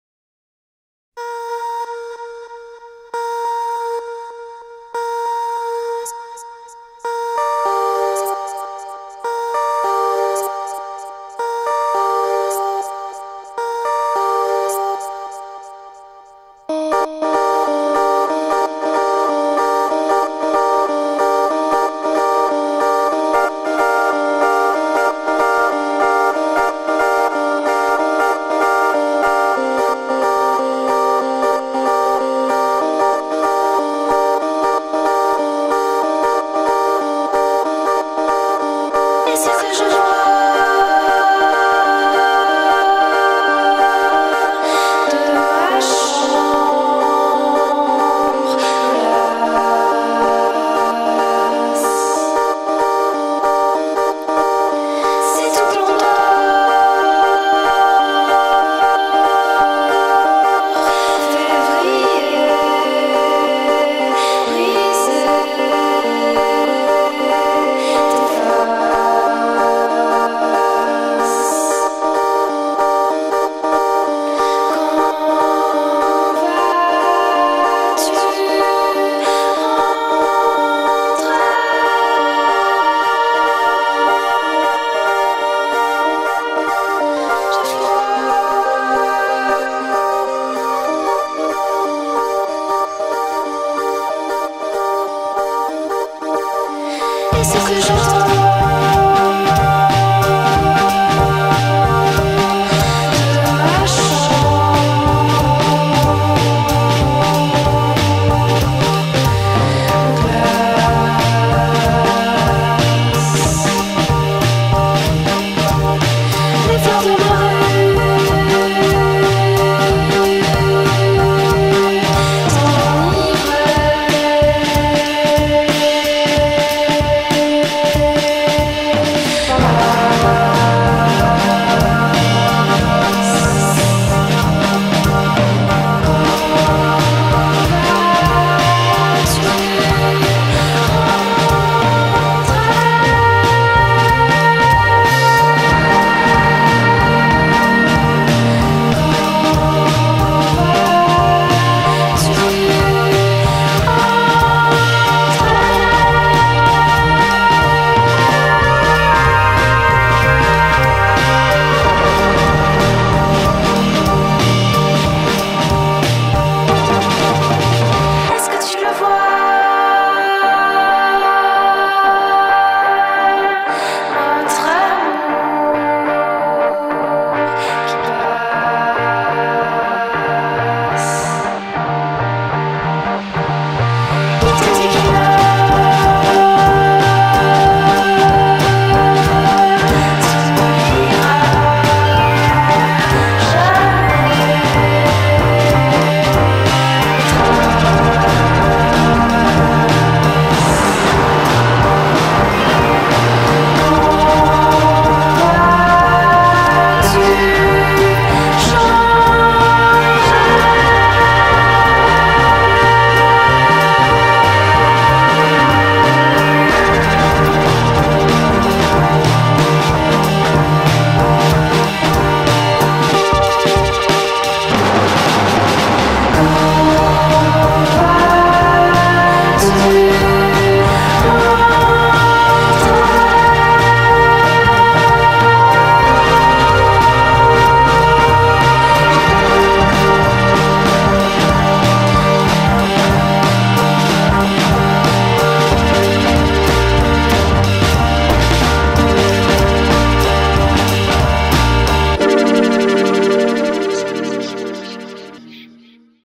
Sunny April skiing in Val D'Isere
Rose and Chloe on the Espace Killy slopes at the beginning of April.